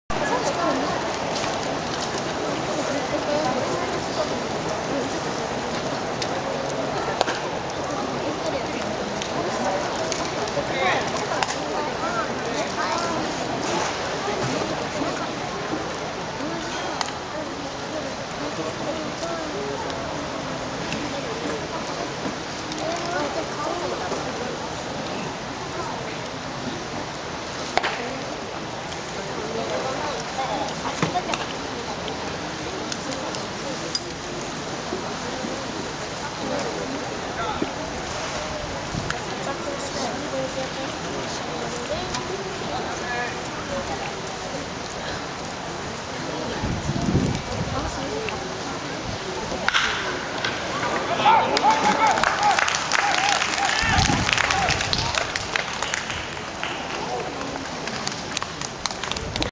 【ヒット！】広島カープ 坂倉 将吾のバッティング_2020052901
マツダスタジアムでの紅白戦、広島カープ 坂倉 将吾のバッティングです。ライト前ヒット！ピッチャーは九里です。